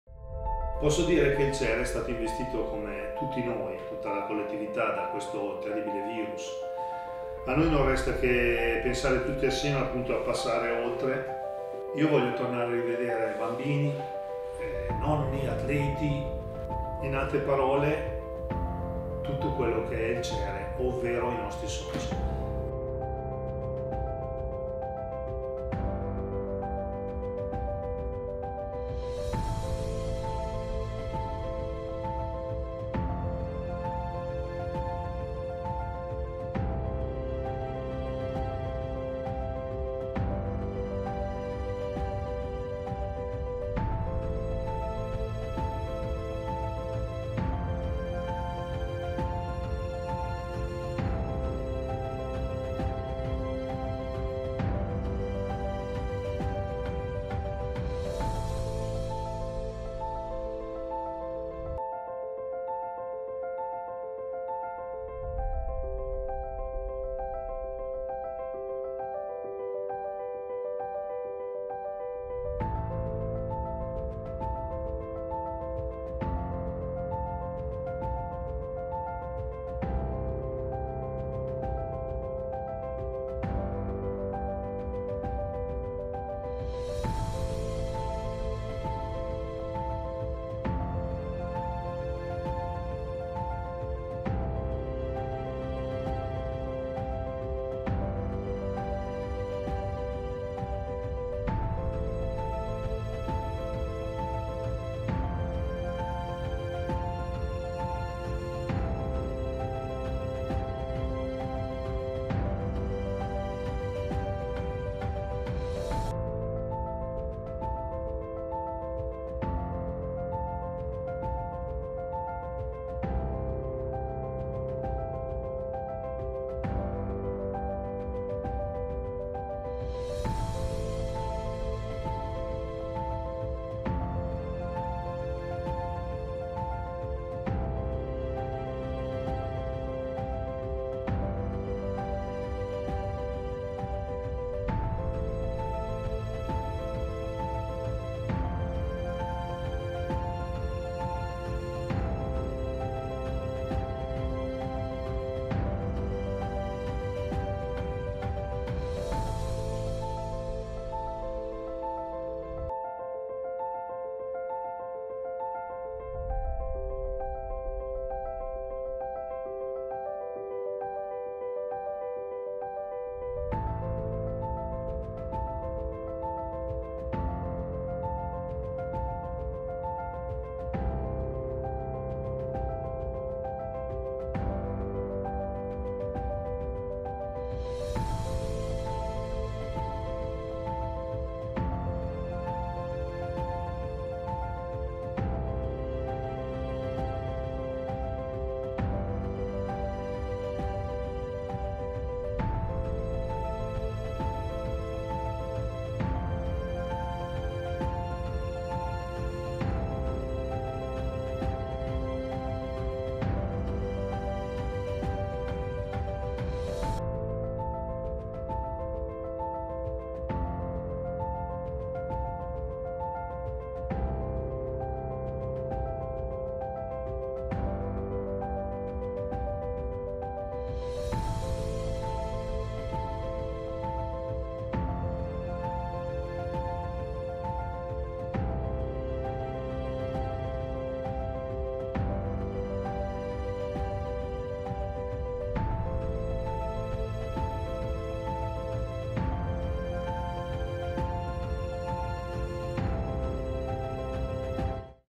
Cere | Il nostro 2020...
Mille le difficoltà e tanti gli ostacoli ma grandissima è stata la voglia di andare avanti.
Ci lasciamo alle spalle un 2020 complicato ma siamo riusciti ancora una volta a superare le difficoltà.
Questo è un omaggio per tutti quelli che hanno mantenuto vivo il nostro circolo...grazie a tutti.
Buon Natale e un augurio sincero per un 2021 di felicità e rinascita